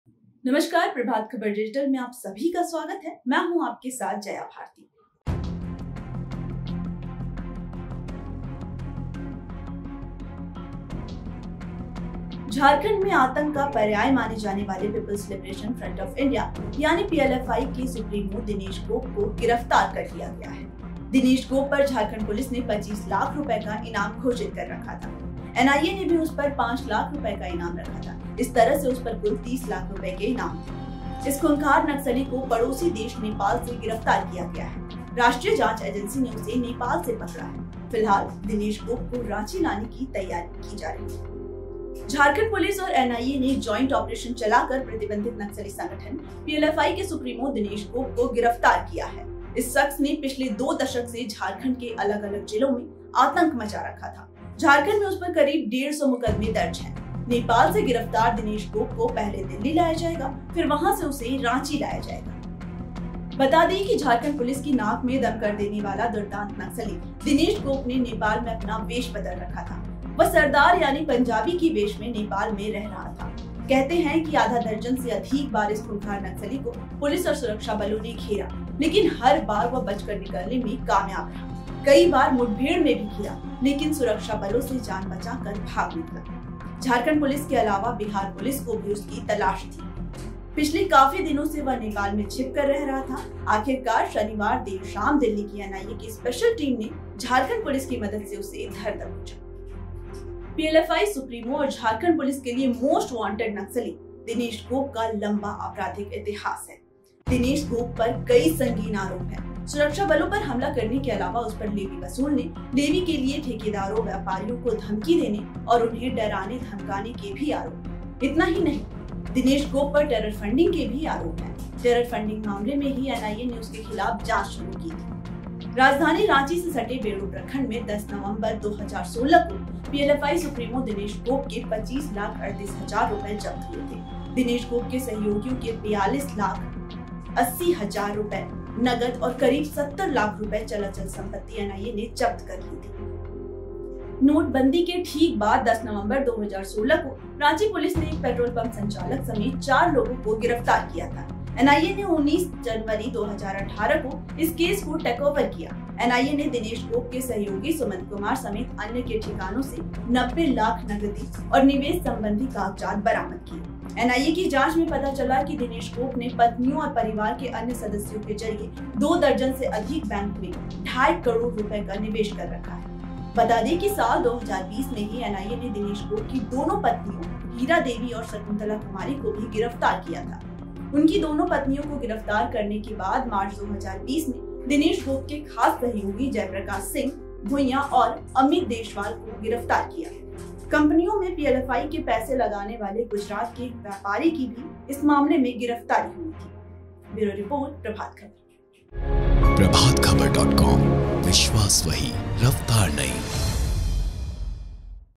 भेष बदलकर नेपाल में छिपा था PLFI सुप्रीमो, दिनेश गोप को NIA ने किया गिरफ्तार | Prabhat Khabar
झारखंड के एक दुर्दांत नक्सली को पड़ोसी देश नेपाल से गिरफ्तार कर लिया गया है. इसने दो दशक से पुलिस की नाक में दम कर रखा था. सूबे के अलग-अलग जिलों में आपराधिक और नक्सली गतिविधियों को अंजाम देता था. पुलिस और राष्ट्रीय जांच एजेंसी (एनआईए) को इसको गिरफ्तार करने में कई साल लग गये. आखिरकार पुलिस और एनआईए के संयुक्त प्रयास से नेपाल में पकड़ा गया. इस शख्स का नाम दिनेश गोप है. आखिर कौन है दिनेश गोप, जिसकी गिरफ्तारी से पुलिस ने राहत की सांस ली है.